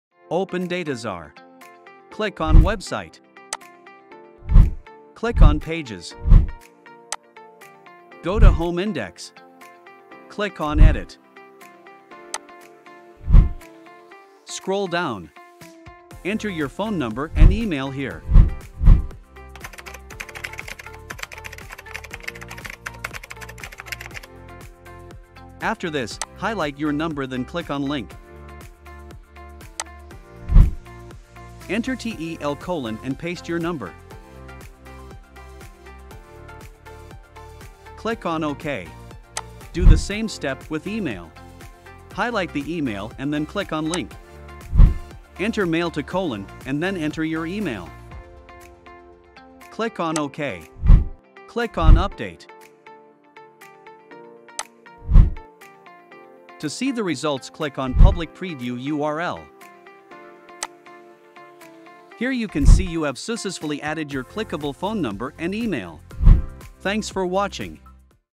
Creating Clickable Email and Phone Links for Your Site
In this video, we'll show you how to create clickable email and phone links for your website. We'll walk you through the step-by-step process of adding these links to your website using HTML, so you can provide a seamless user experience for your visitors. Whether you're a business owner, freelancer, or just want to improve your website's functionality, this tutorial is for you.

Create your own website, build your customer list and start email marketing today - by going